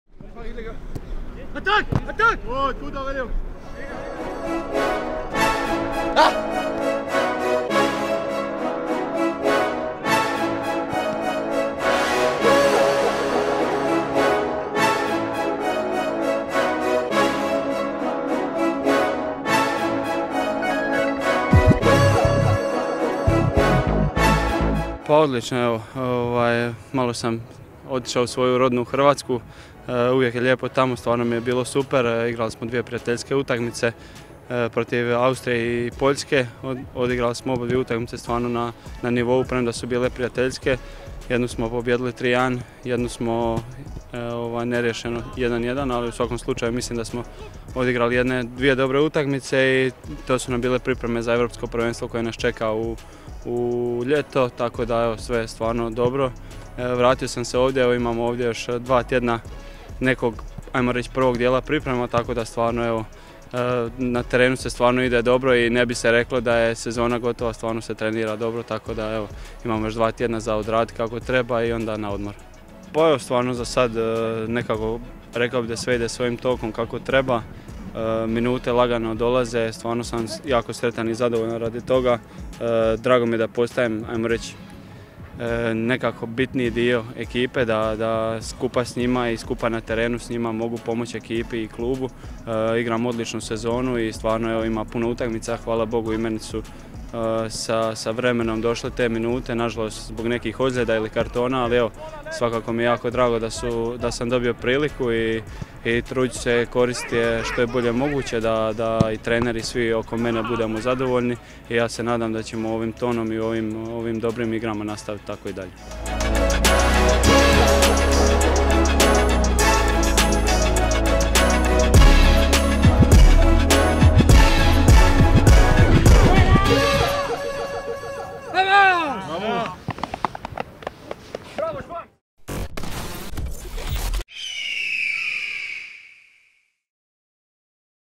"In diesem Rhythmus weitermachen" I Intensive Einheit für Smolcic & Co.
Die Adlerträger starteten mit einer intensiven Einheit in die vorletzte Woche vor der Winterpause. Nach dem Training haben wir mit unserem Verteidiger Hrvoje Smolcic über seinen Einsatz bei der kroatischen U21 und die Hinrunde gesprochen.